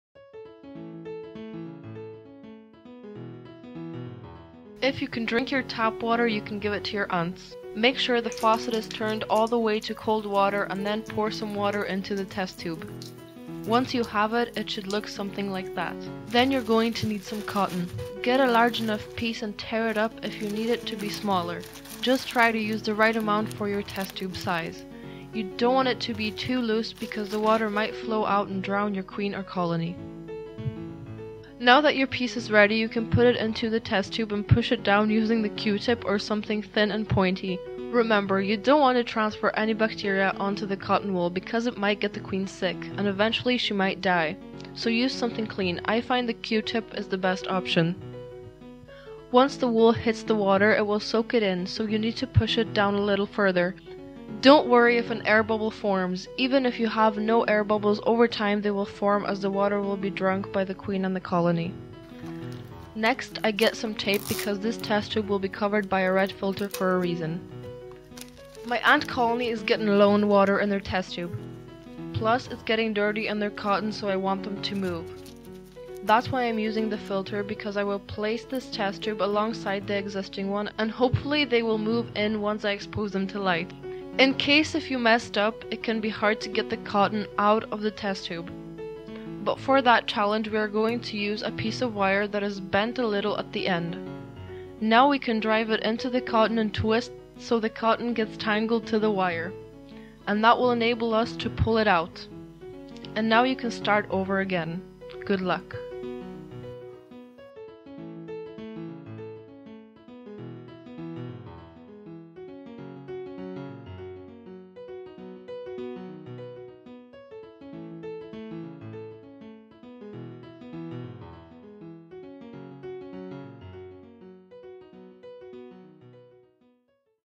How To Make A Test-Tube Setup | ANTS IRELAND
In this video you will learn how to make a test-tube setup in a minute.